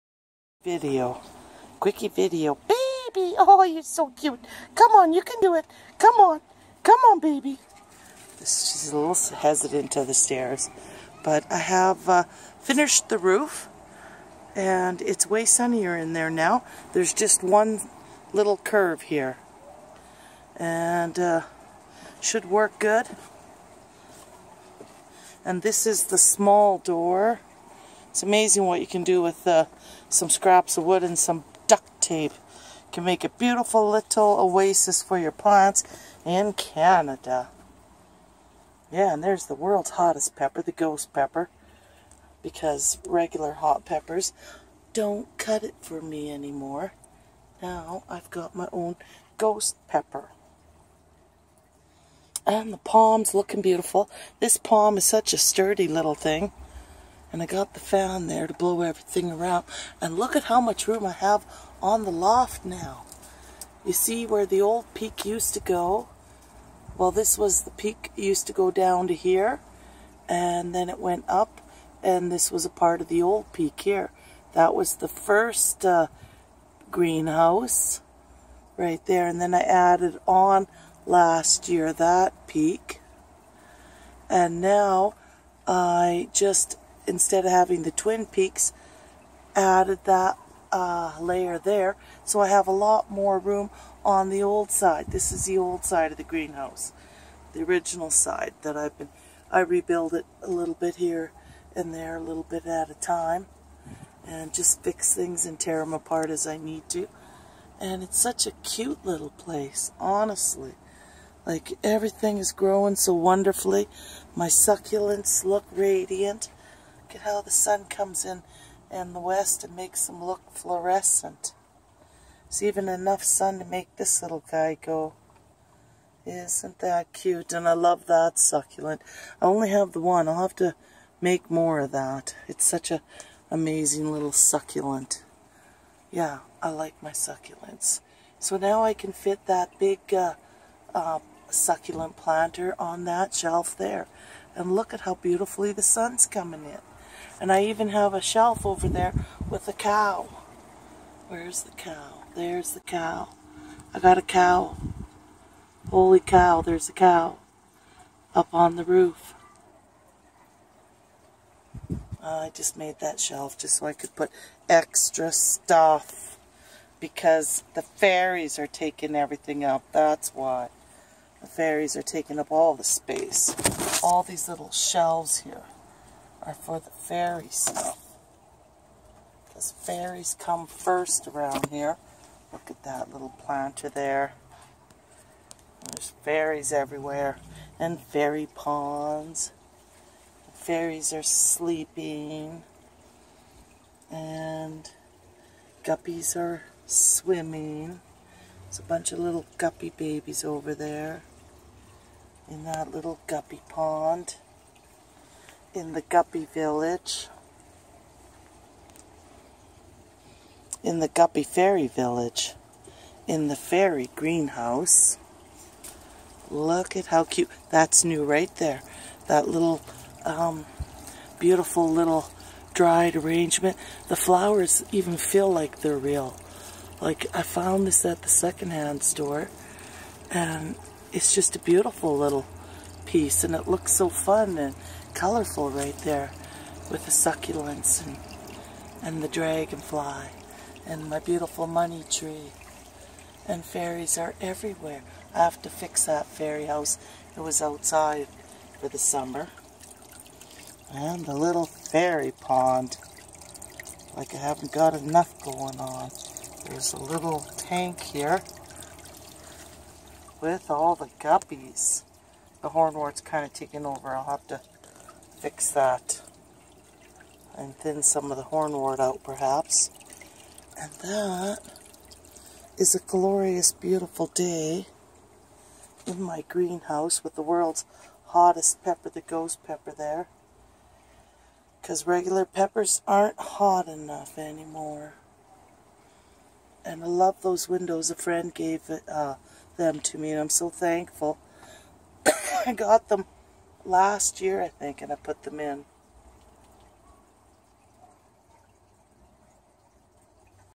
Update on Greenhouse in Canada!
I have expanded the roof on old side of greenhouse and now I have more space on upper loft.  Succlents are easy to grow over winter in greenhouse.  I love Succlents!